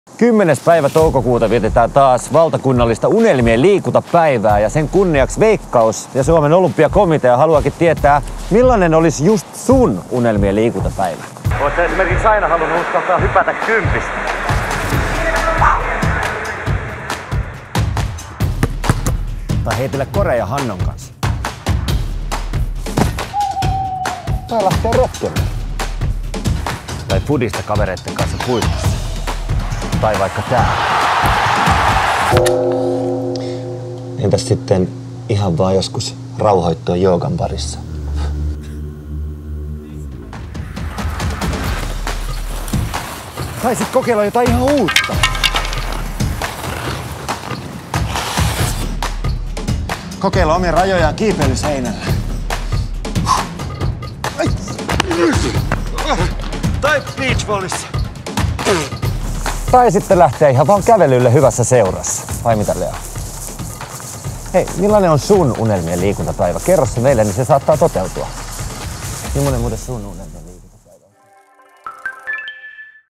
Unelmien liikuntapäivä 2019 | Veikkaus
Unelmien liikuntapäivää vietetään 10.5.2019. Veikkaus ja Suomen olympiakomitea haluavat tietää minkälainen olisi juuri sinun unelmien liikuntapäivä.

Category
Sports